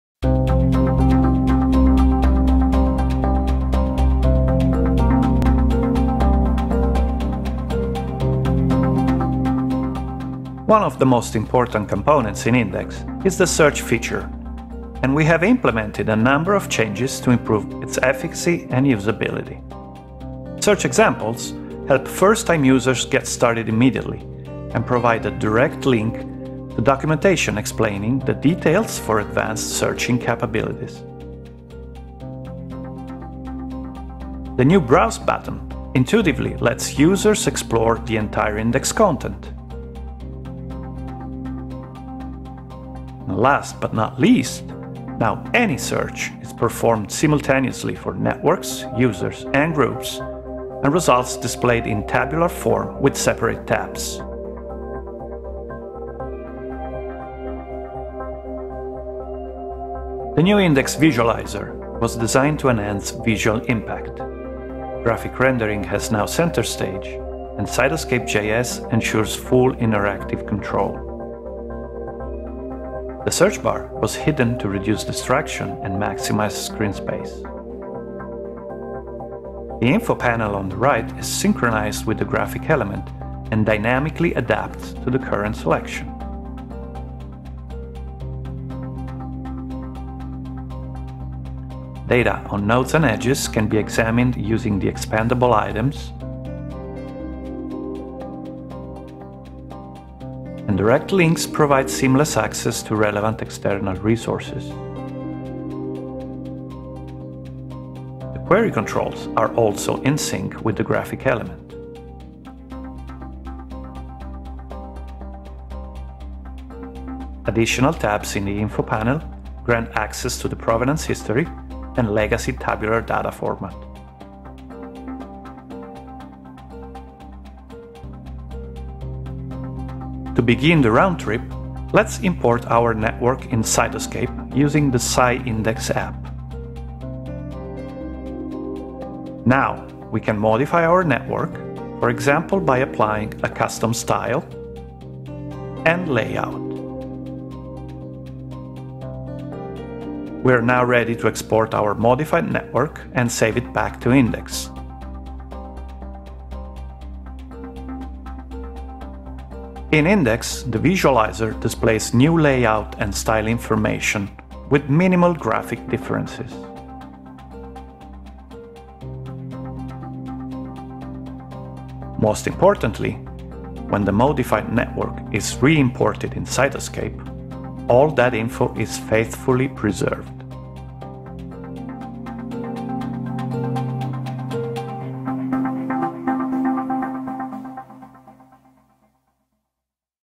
NDEx Video Demo - June 2016
This video demo describes major improvements in the NDEx Web UI and was generated as part of the presentation for the annual ITCR PI Meeting 2016.